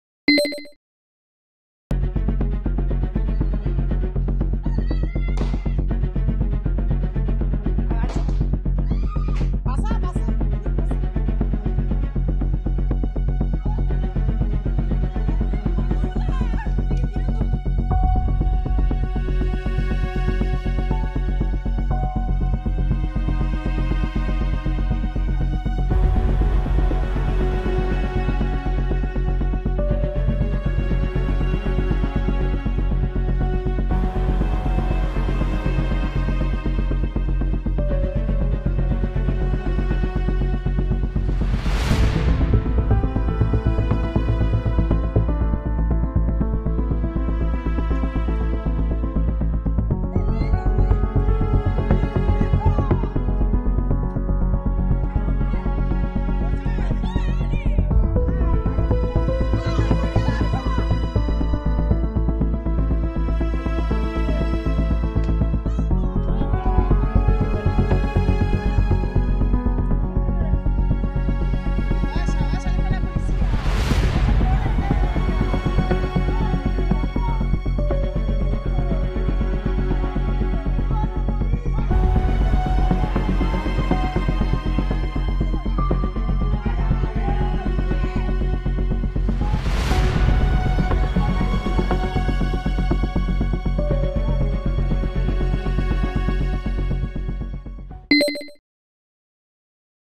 Villa Tesei: dos bandas narco se enfrentaron a los tiros a metros de un Jardín de Infantes
Durante el tiroteo, tres personas resultaron heridas. Tras la balacera, la Policía allanó una vivienda y detuvo a un delincuente.